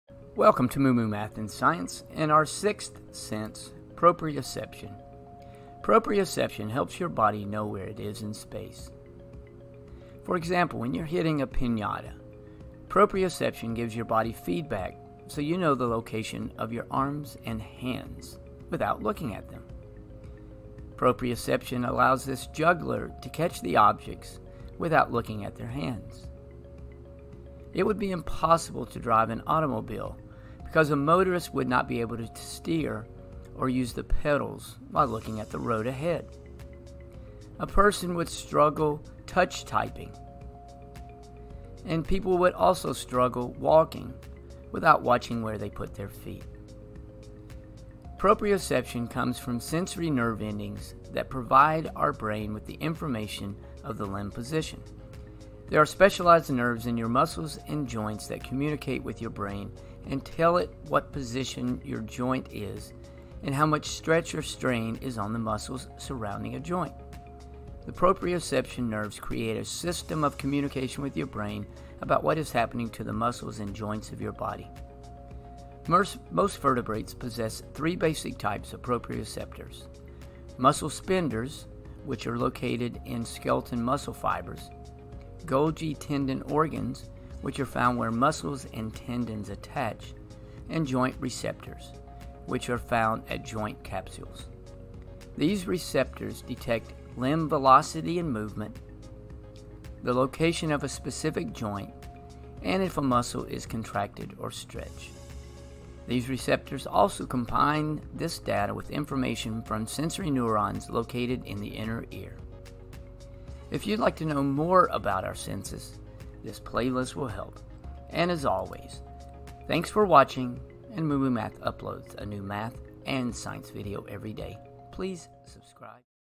Proprioception- Is it your 6th Sense?
The 6th sense- Proprioception
Proprioception helps your body know where it is in space.
For example, when you are hitting a pinata, proprioception gives your body feedback so that you know the location of your arm and hand.
Proprioception allows this juggler to catch the objects without looking at their hands.

 it would be impossible to drive an automobile because a motorist would not be able to steer or use the pedals while looking at the road ahead; a person could not touch type and people would not even be able to walk without watching where they put their feet.

Proprioception comes from sensory nerve endings that provide our brain with the information of the limb position. There are specialized nerves in your muscles and joints that communicate with your brain and tell it what position your joint is in and how much stretch or strain in on the muscles surrounding a joint. The proprioception nerves create a system of communication with your brain about what is happening to the muscles and joints of your body.